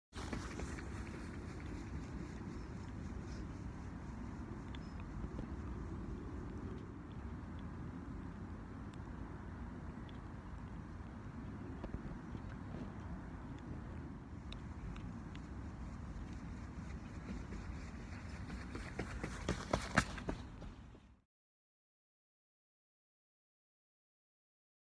Running along Bennerley Viaduct - Marathon training fun!
A short video documenting the fun I had  running across Bennerley Viaduct recently as part of my marathon training. To discover more about my running adventures, head to timsrunningworld.com!